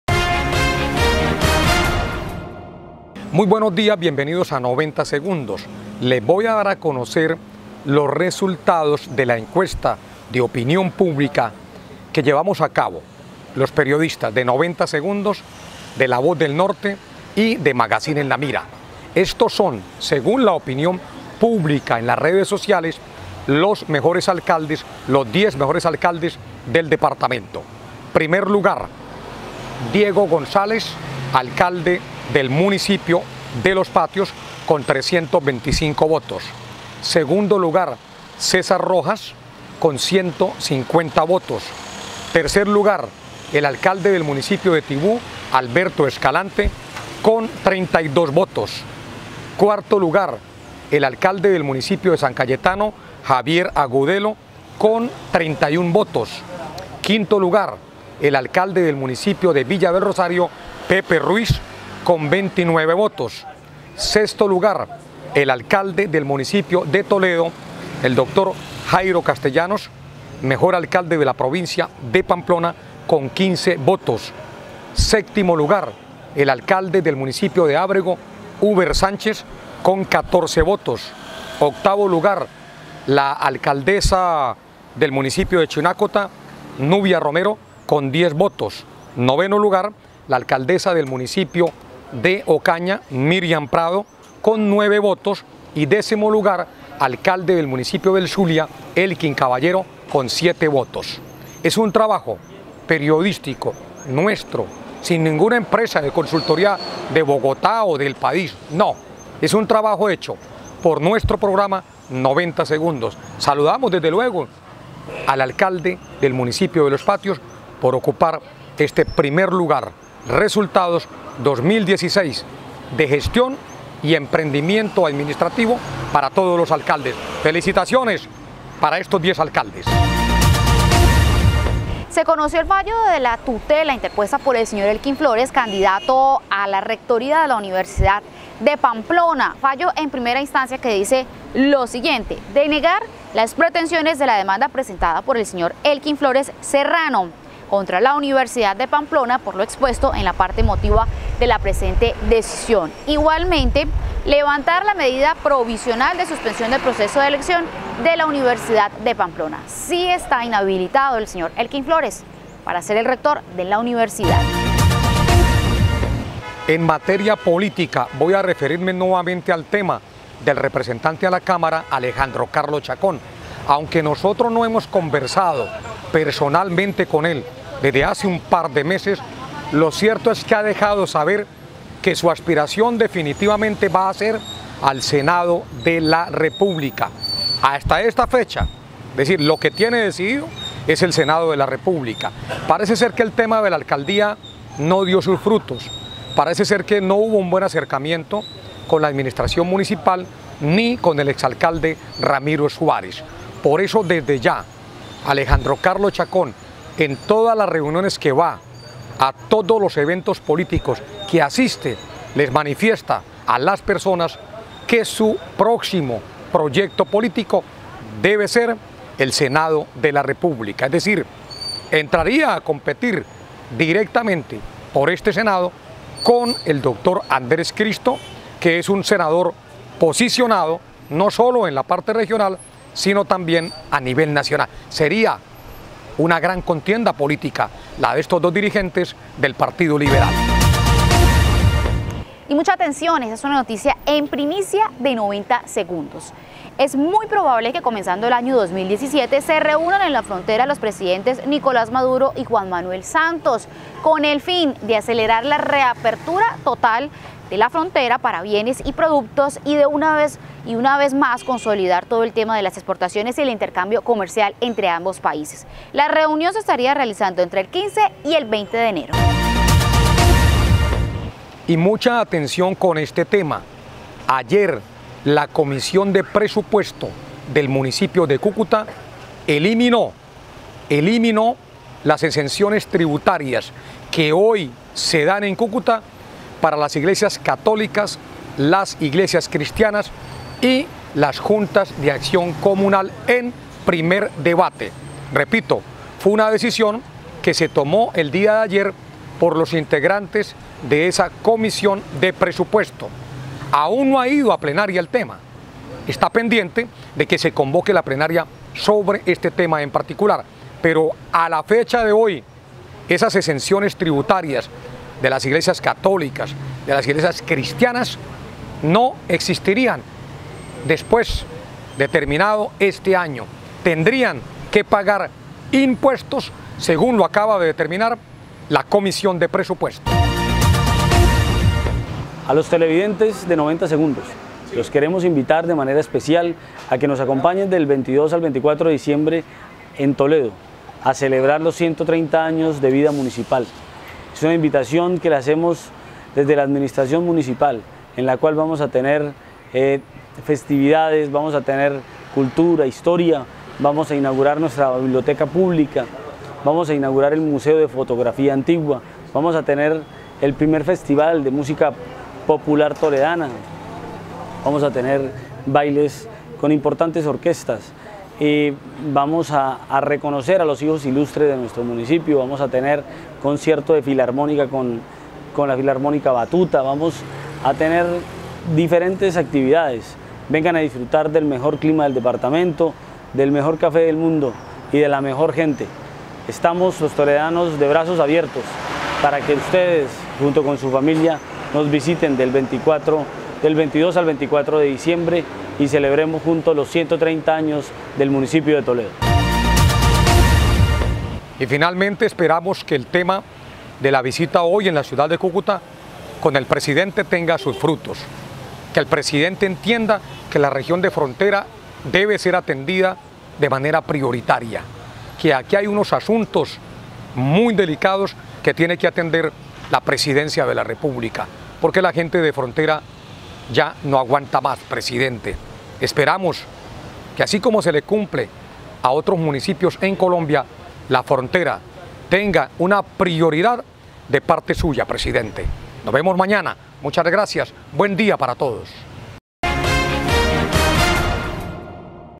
CHACÓN SERÍA CANDIDATO AL SENADO DE LA REPÚBLICA 20 DICIEMBRE
90 SEGUNDOS CON FREDY PARADA
TEMAS:
1. RESULTADOS ENCUESTA ALCALDES 2.016
2. EN FIRME LA INHABILIDAD DE ELKIN FLÓREZ
3. CHACÓN SERÍA CANDIDATO AL SENADO DE LA REPÚBLICA
4. SE ACELERARIA REAPERTURA TOTAL DE LA FRONTERA EN 2.017
5. ELIMINAN EXCENSIONES TRIBUTARIAS EN EL CONCEJO
6. INVITACIÓN A LA CELEBRACIÓN 130 AÑOS DE TOLEDO
7. SE LE PEDIRÁ PRIORIDAD AL PRESIDENTE PARA CÚCUTA
FACEBOOK: Magazín En La Mira
YOUTUBE:Magazín En La Mira